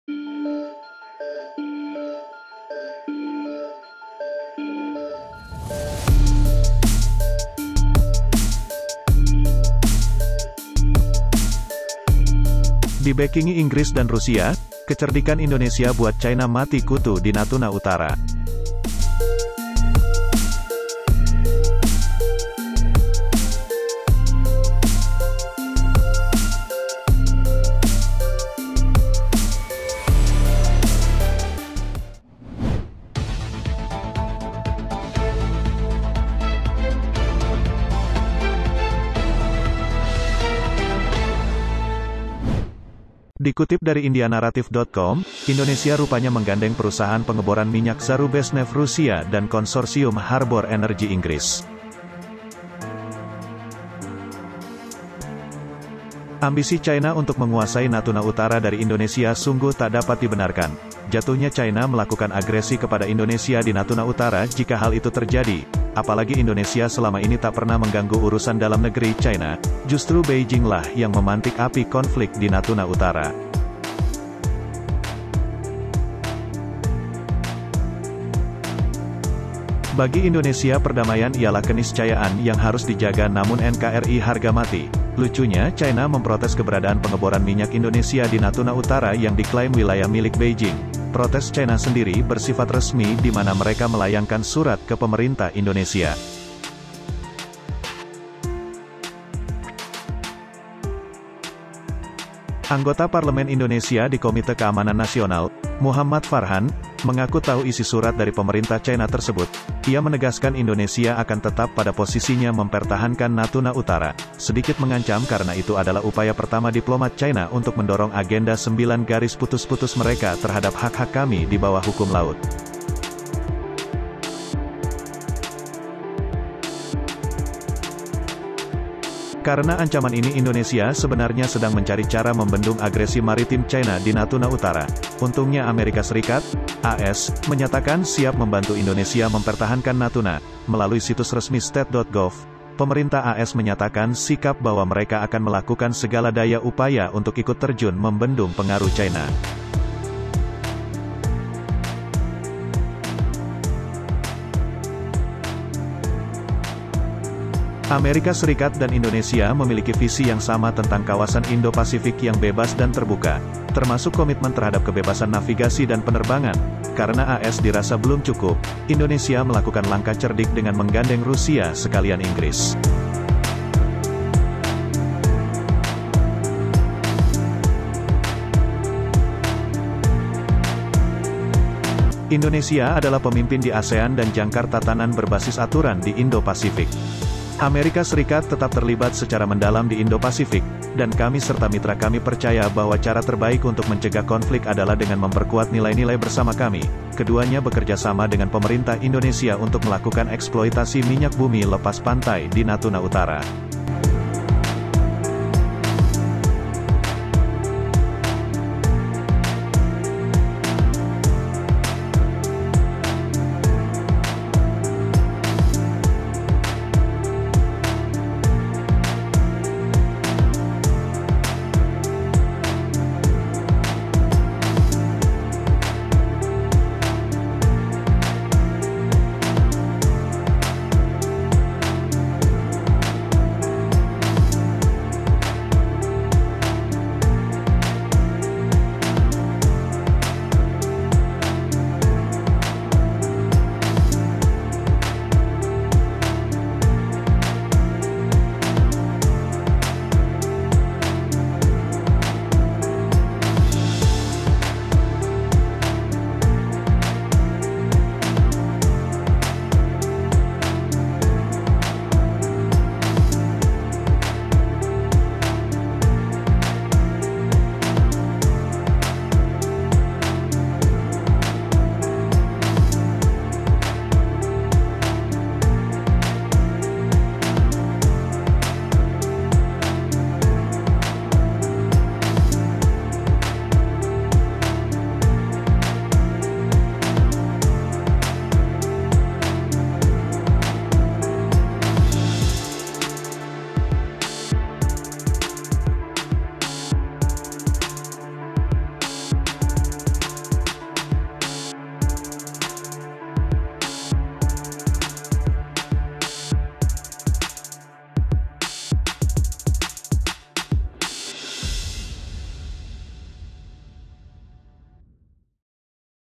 CHINA GEMETAR !! INGGRIS DAN RUSIA SIAP MENJADI PELINDUNG INDONESIA DI NATUNA
Indonesia memiliki visi yang sama tentang kawasan Indo-Pasifik yang bebas dan terbuka, termasuk komitmen terhadap kebebasan navigasi dan penerbangan.Karena AS dirasa belum cukup, Indonesia melakukan langkah cerdik dengan menggandeng Rusia sekalian Inggris

🇮🇩🇲🇾 Hai Sobat Info Rakyat! Apa kabar hari ini?
🎞 Video baru hampir tiap hari. Aktifkan ikon lonceng ya.